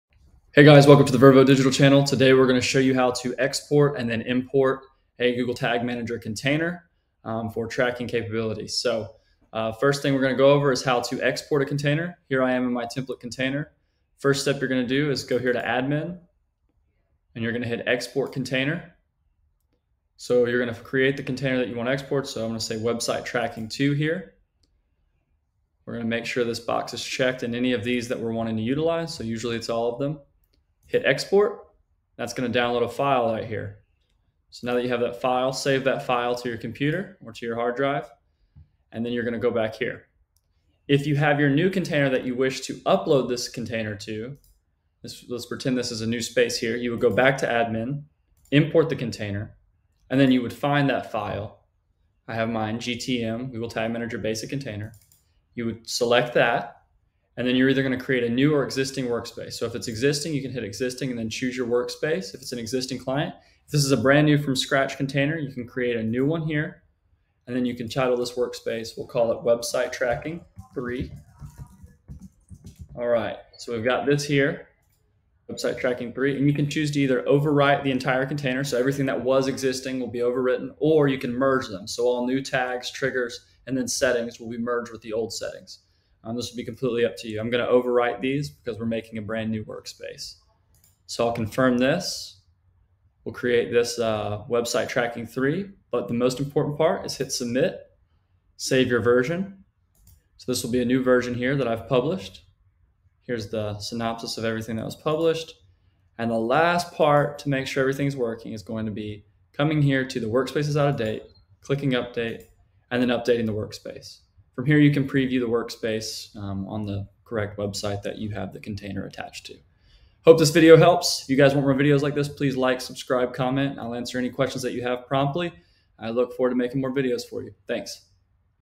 Import and Export Google Tag Manager Containers (GTM) *2023*
In this video you will learn how to export your Google Tag Manager container including tags, triggers, versions, etc. You will also learn how to import the settings into an existing container.

This is a time saving method that will allow you to quickly onboard new clients.

Please let us know if you have any questions in the comments.